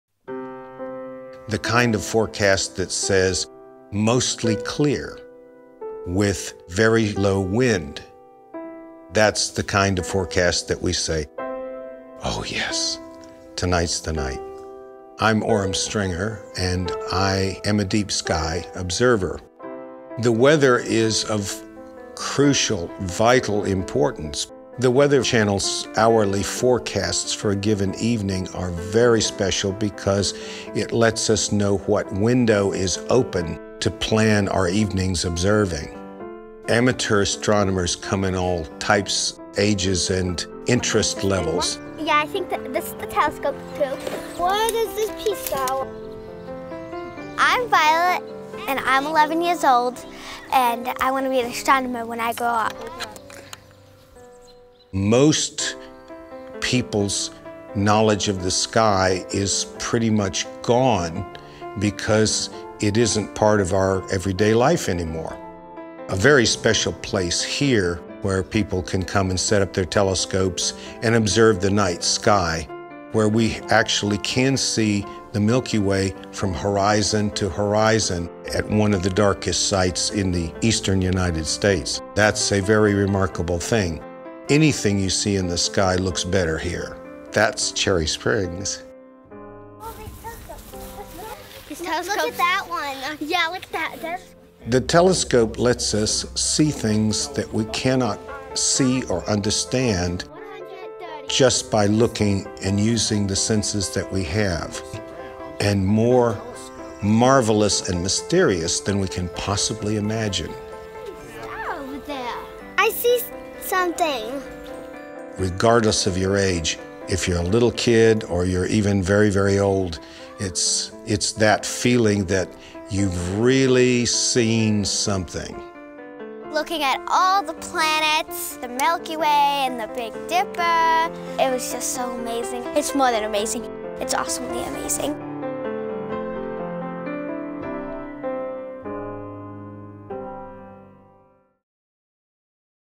The most beautiful stargazing location on Earth
Hundreds of amateur astronomers gather at Cherry Springs in Pennsylvania to catch a glimpse of the stars and the Milky Way. They turn to The Weather Channel forecast to find clear skies, low wind, and low humidity for the best views of the cosmos.